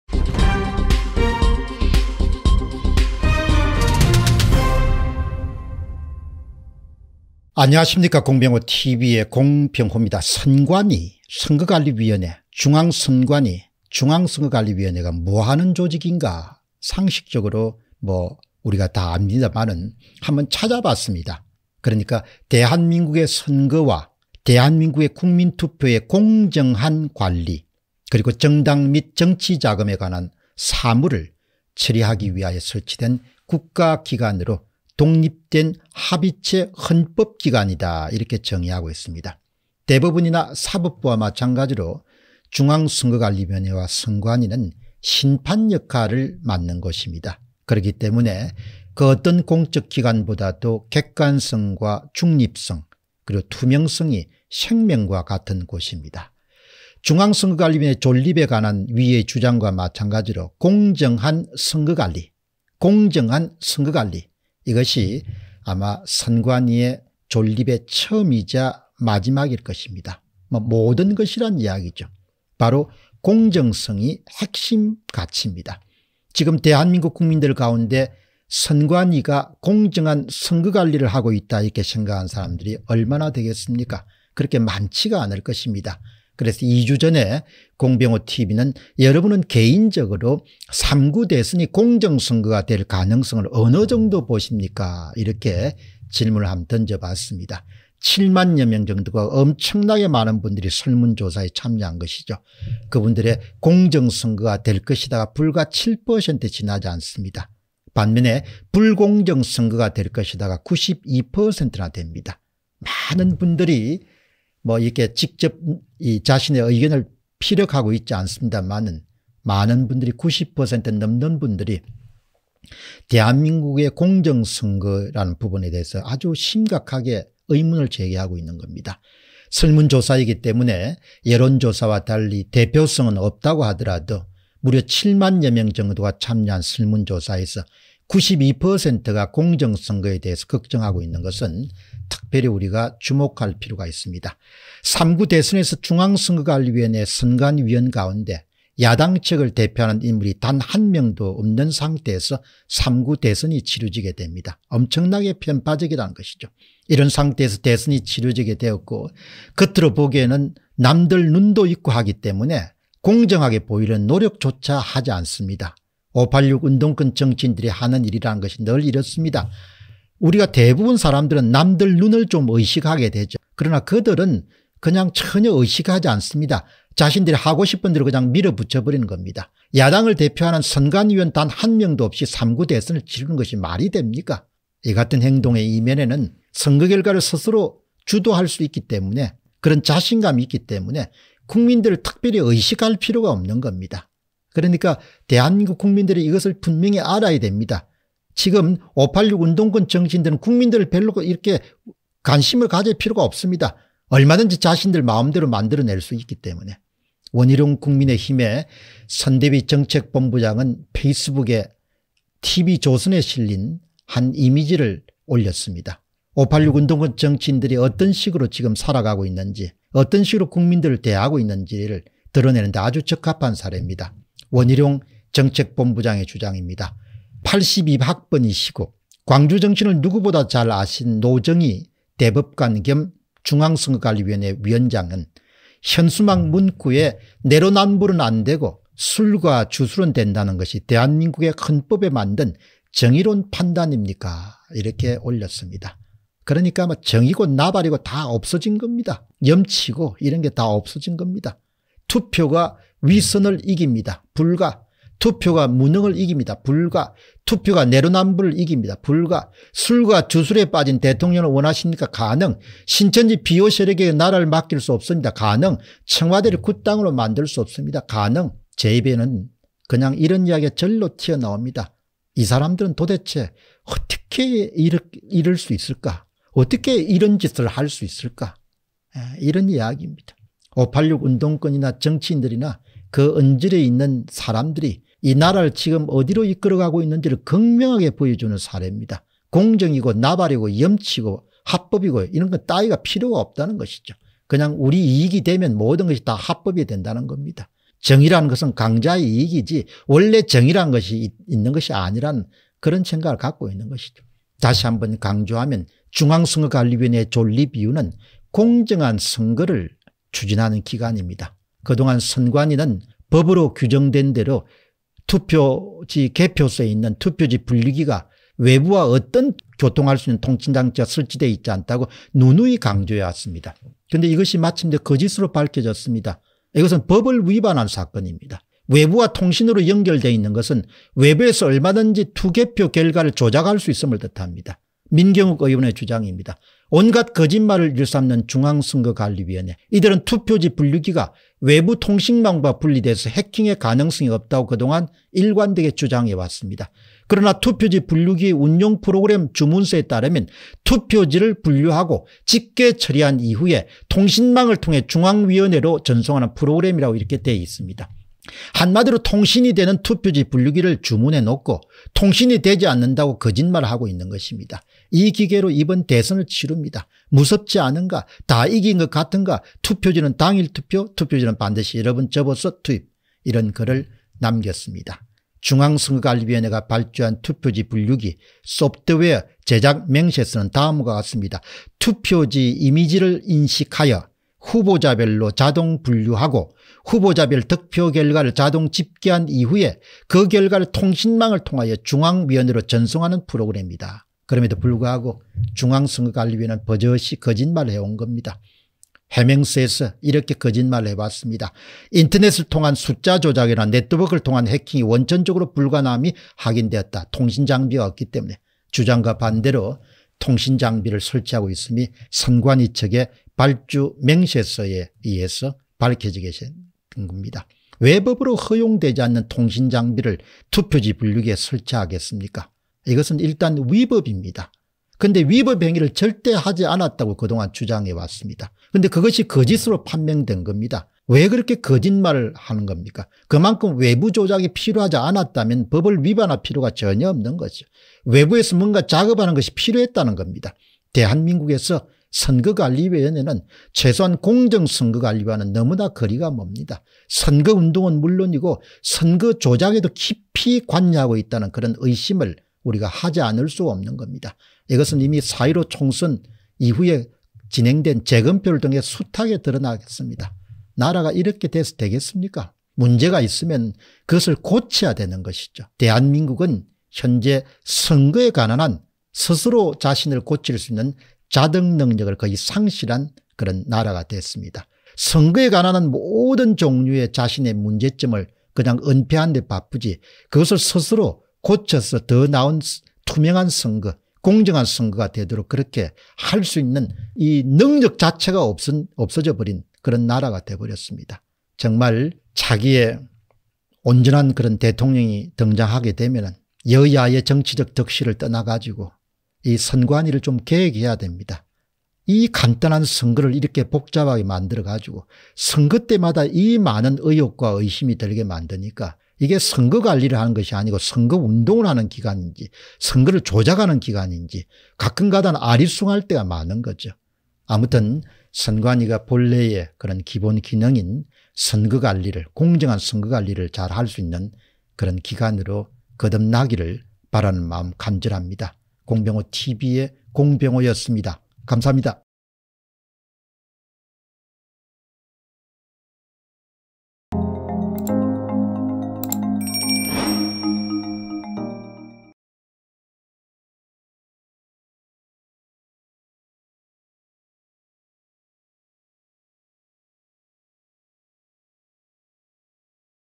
대한민국 선관위 / 선거운동? 선거조작? / 법으로는 '공정한 선거관리'를 위한 조직이라지만 [공병호TV]
Bitcoin : 3MTW849AkP2ftLeSK2teZHMmzLu7Z83s9U